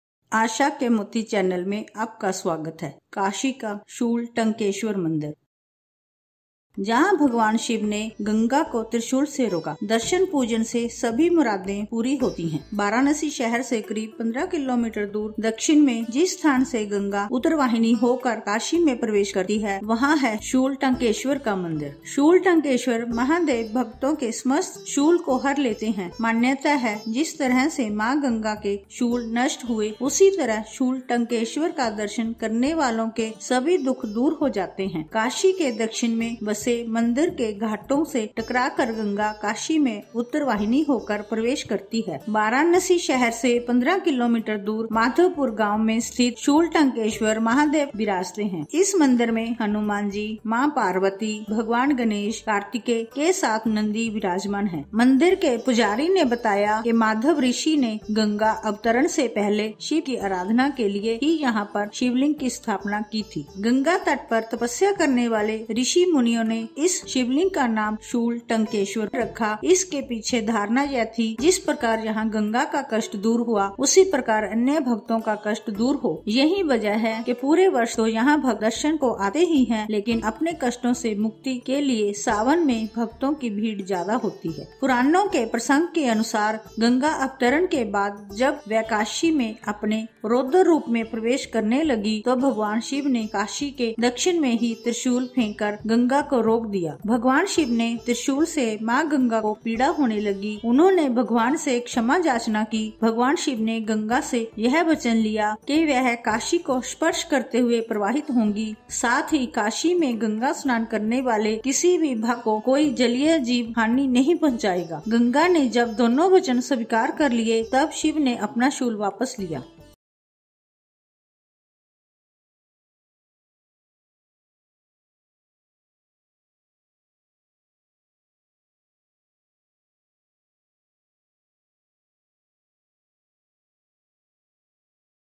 Shooltankeshwar Mahadev | शूलटंकेश्वर महादेव, भगवान शिव ने यहीं त्रिशूल से रोका था गंगा के वेग को
काशी के गंगा तट पर स्थित शूलटंकेश्वर महादेव मंदिर श्रद्धालुओं के आस्था का केंद्र है। दूर-दूर से लोग यहां दर्शन पूजन के लिए आते हैं। शिवरात्री और सावन के अलावा छट पूजा पर भी यहां खूब भीड़ उमड़ती है।
मान्यता है कि गंगा अवतरण के समय भगवान शिव को काशी की सुरक्षा की चिंता सताने लगी। उन्होंने यहीं अपना त्रिशूल गाड़कर गंगा के वेग को रोका था। इसलिए इनका नाम शूलटंकेश्वर पड़ा। द्वापर में ब्रह्मा ने यहां वीरेश्वर महादेव के शिवलिंग की स्थापना की। दक्षिण दिशा में विराजमान शूलटंकेश्वर विपत्तियों से इस नगरी की रक्षा करते हैं।
गंगा तट पर तपस्या करने वाले  ऋषि-मुनियों ने इस शिवलिंग का नाम शूलटंकेश्वर रखा। इसके पीछे धारणा यह थी कि जिस प्रकार यहां गंगा का कष्ट दूर हुआ उसी प्रकार अन्य भक्तों का कष्ट दूर हो। यही वजह है कि पूरे वर्ष तो यहां भक्त दर्शन को आते ही हैं लेकिन अपने कष्टों से मुक्ति के लिए सावन में भक्तों की भीड़ ज्यादा होती है।